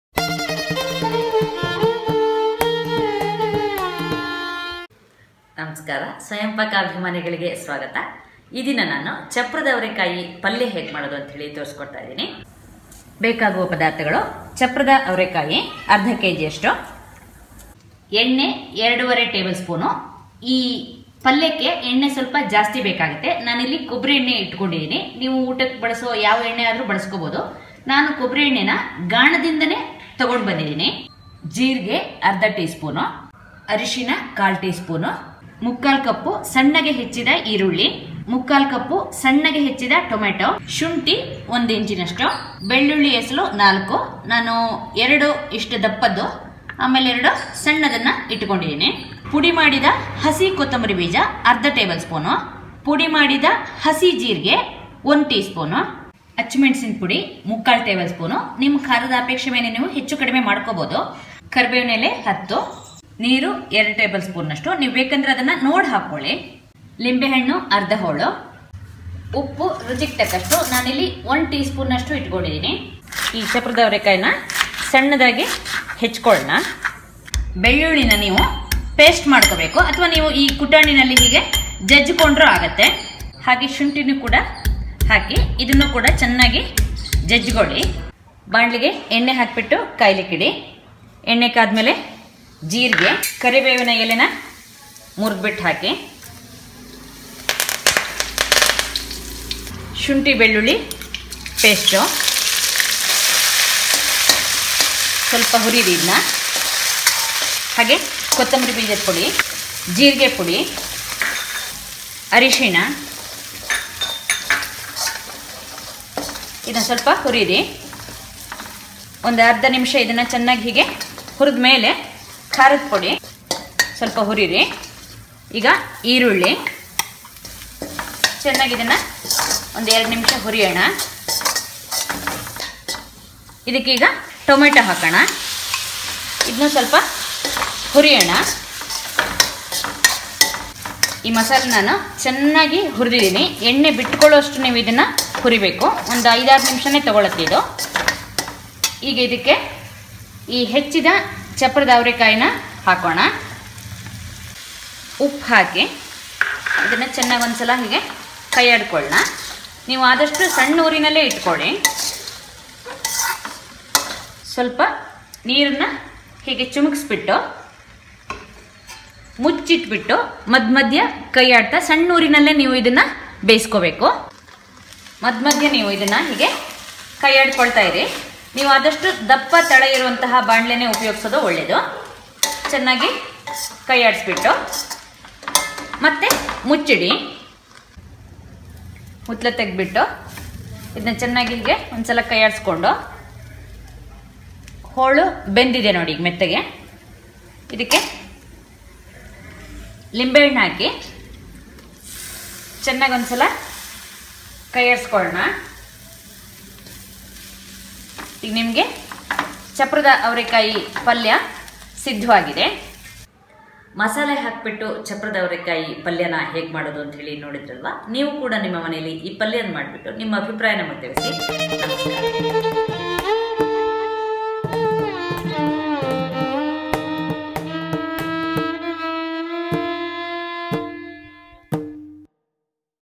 chapparada avarekai palya |ಚಪ್ಪರದ ಅವರೆಕಾಯಿ ಪಲ್ಯ |Indian broad beans curry | chapparadavarekayi
ಚಪ್ಪರದವರೆಕಾಯಿ, easy to make chapparada avarekai palya . masale palya which is vegan and glutenfree
Ingredients:
2.5 tbsp oil
1/2 tbsp jeera/cumin seeds
1/4 tsp turmeric powder
1/2 kg  chapparada avarekayi / flat beans
3/4 cup finely chopped onion
3/4 cup finely chopped tomato
1 inch fresh ginger
4 garlic cloves
1/2 tsp dania powder/coriander seeds powder (raw)
1 tsp jeera powder (raw)
3/4 tbsp red chilli powder
10 curry leaves
around 2 tbsp water
juice of half a lemon /lime
1 tsp salt or salt to taste